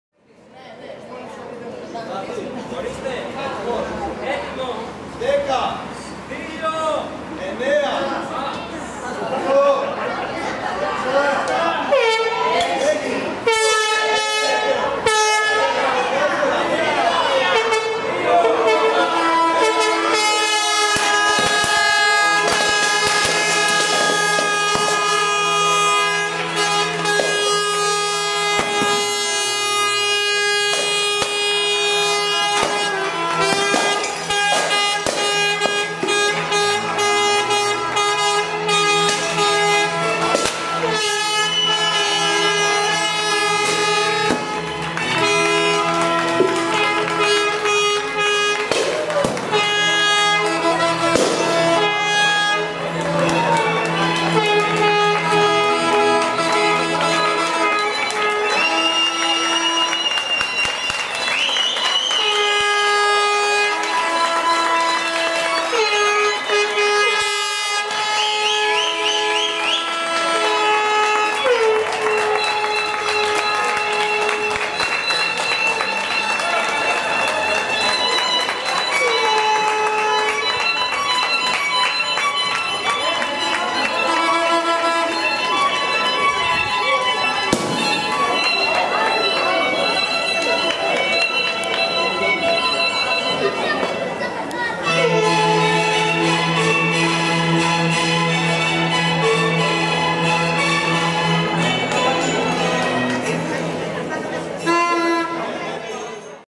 Άκτιος - Πάσχα 2013 - Μπότηδες
Για όσους δεν κατάφεραν να είναι μαζί μας, έγινε η αναβίωση του κερκυραϊκού εθίμου των "Mπότηδων". Πάρτε μια μικρή ιδέα ... από την Πρώτη Ανάσταση

Βίντεο/φωτογραφίες: Ilias Florakis